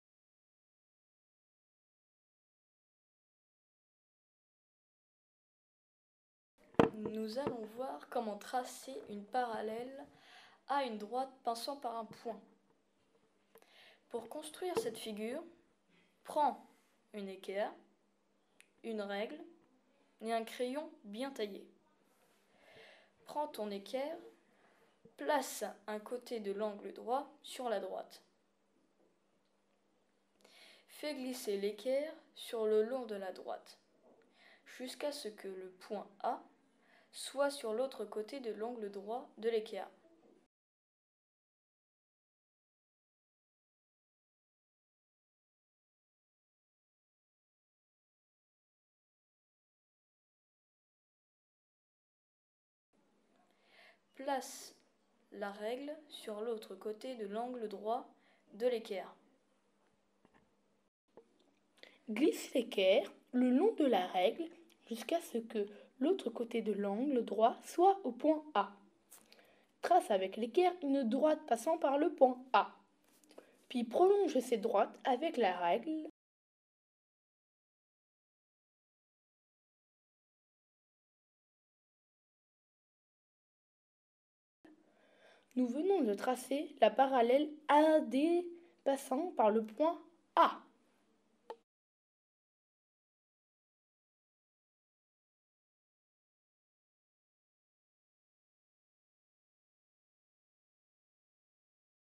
Tracer une parallèle à une droite passant par un point
Vidéo expliquant comment tracer une parallèle à une droite passant par un point donné avec une règle et une équerre.